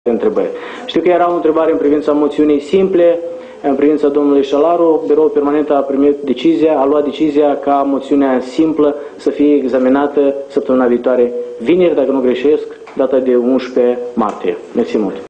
Declarațiile lui Andrian Candu după ședința biroului permanent al Parlamentului ...
Titlul evenimentului: Declarațiile lui Andrian Candu după ședința biroului permanent al Parlamentului din 4 martie 2016